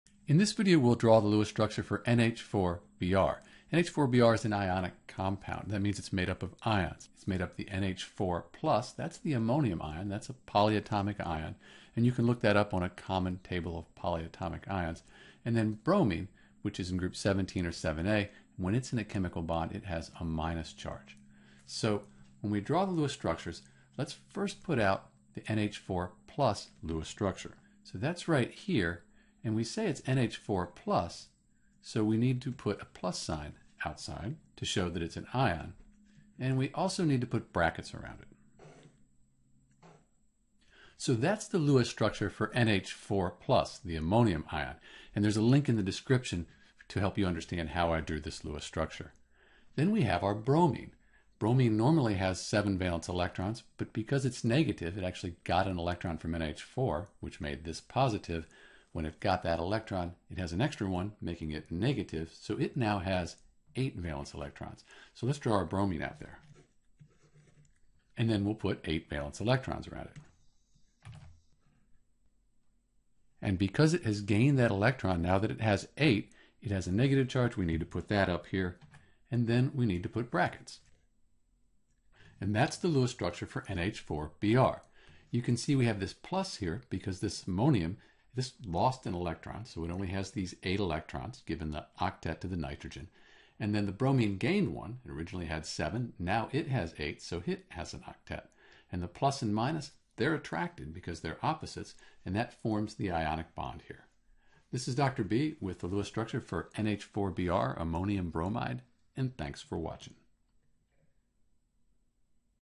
How to draw the NH4Br Lewis Dot Structure (Ammonium bromide)
For NH4Br we have an ionic compound and we need to take that into account when we draw the Lewis Structure.  We’ll first draw the Ammonium ion (NH4+) and put it in brackets with its charge on the outside.   for how to write the Lewis Structure for NH4+ .

Next, we'll draw the Lewis Structure for the Br- ion.

The Lewis structure of (NH4)Br is also called Ammonium bromide.